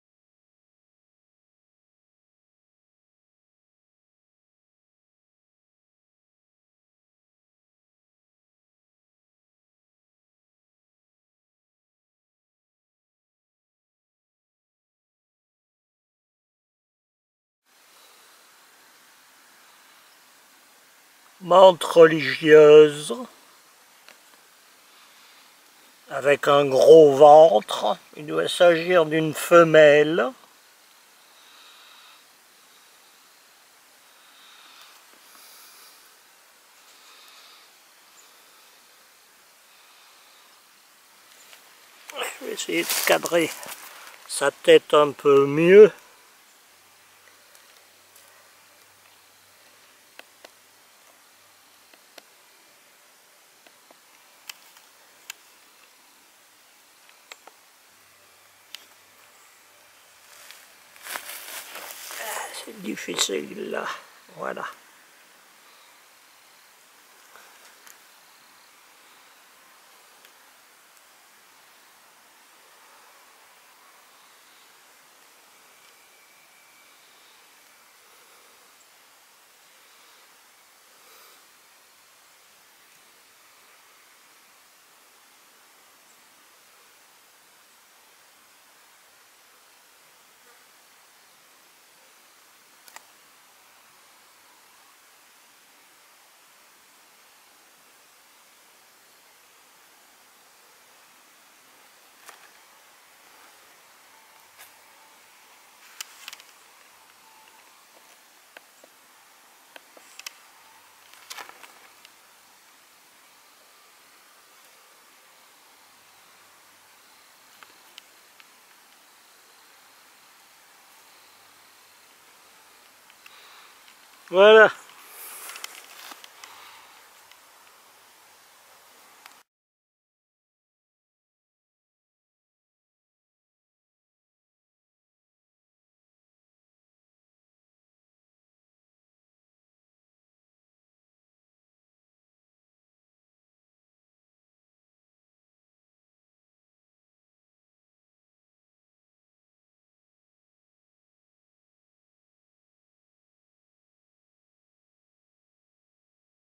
Mante religieuse 25 octobre 2024
Mante religieuse 25 octobre 2024 by @K47CB. La Mante religieuse chasse à l'affut dans la végétation. Verte dans l'herbe verte, elle prend une couleur grise ou beige dans les herbes sèches. Grâce à ses pattes ravisseuses bardées d'épines, la Mante capture ses proies plus vite que son ombre, c'est l'un des mouvements les plus rapides du règne animal.